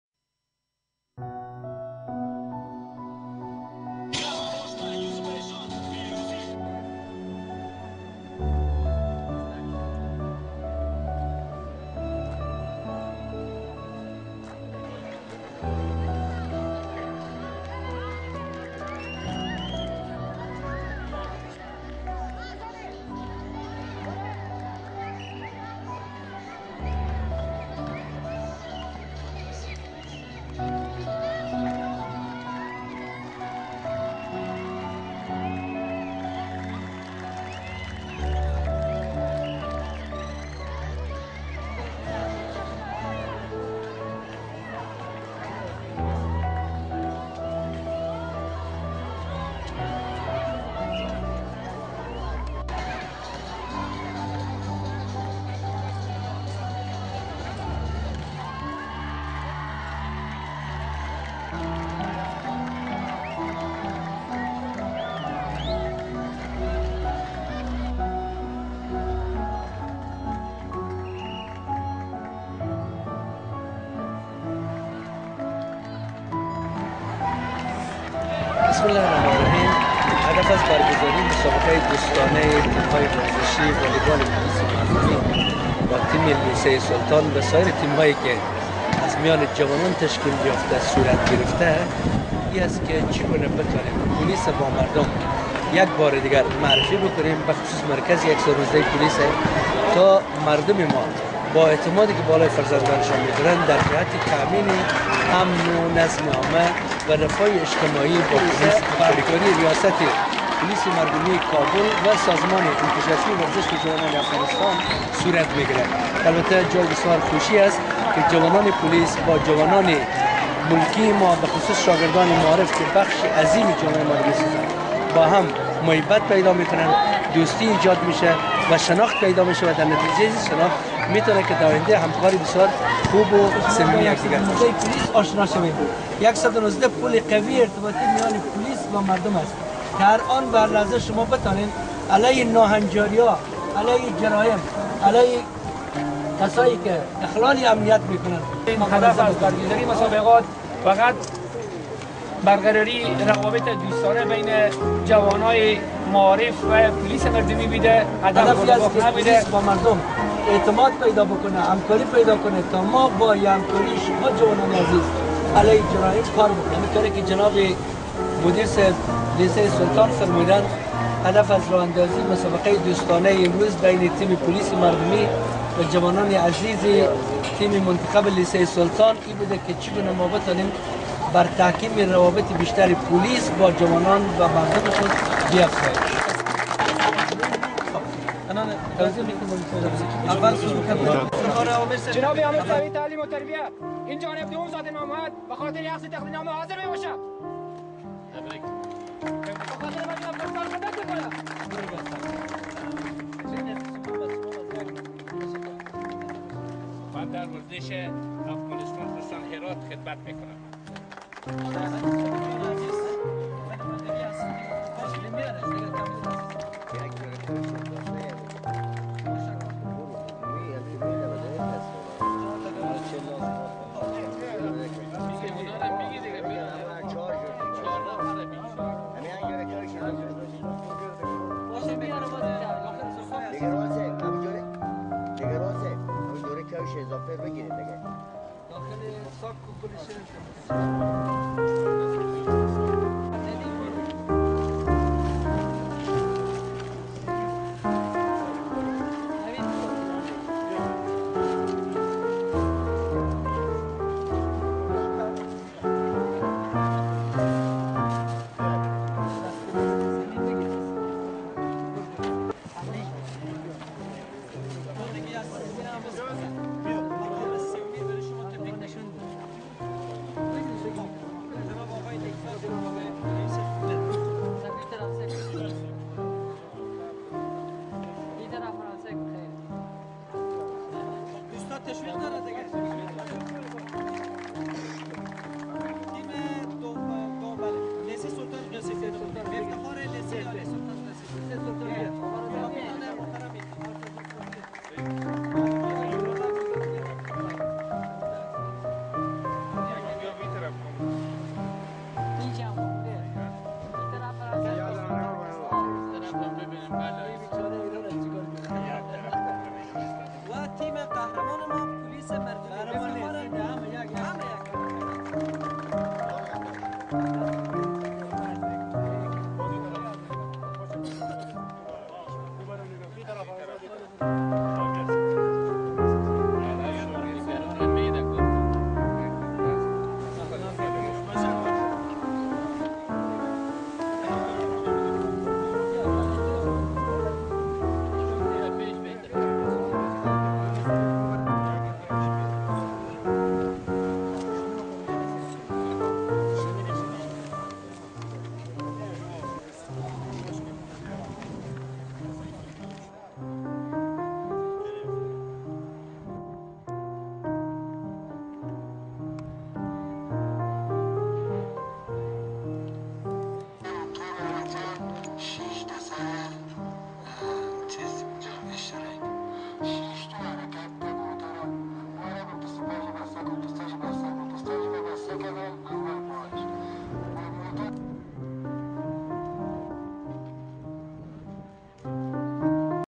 43rd herat volleyball چهل و سومین دوره مسابقات والیبال در ولایت هرات
هدف از برگزاری این مسابقات به منظور نزدیک ساختن ساکنین محل با پولیس و پولیس با مردم میباشد   وبه شماره ۱۱۹ پٖولیس تماس گرفته تا از جرایم و حوادث در سرتاسر افغانستان جلوگیری شود.
 چهل و سومین دوره مسابقات والیبال به تاریخ ۱۰ الیٰ ۱۳ سبتمبر ۲۰۱۵ در ولایت هرات برگذار گردید که در این رویداد ورزشی به تعداد چهار تیم شرکت ورزیده بودند. که تیم والیبال پولیس مردمی توانست مقام اول را کسب نماید و تیم والیبال سلطان غیاث غوری  مغام دوم را از آن خود کرد.. که در ختم مسابقات کپها و مدالهای ؛حکم های قهرمانی و تقدیرنامه ها برای ورزشکاران توزیع گردید همچنان  برای مهمانان عزیز فلم مستند ۱۱۹ به نمایش گذاشته شد وغذای چاشت برای حاضرین تهیه گردید.
The aim of these events is to close the residents with the police and the police with people.
The police emergency number 119 is to prevent crimes and incidents across the country.

43rd periods of Volleyball Championships from 10 to 13 September 2015 held in Herat Province who attended the event with four teams. The Police E Mardumi Volleyball team won the first place and the Sultan Gheyas uldin Ghori high School Volleyball team won the second place in this competition at the end of competition Sentence Championships, cups and medals were distributed to athletes, the launch was prepared for Attendees. And the 119 police documentary Exhibited for the guests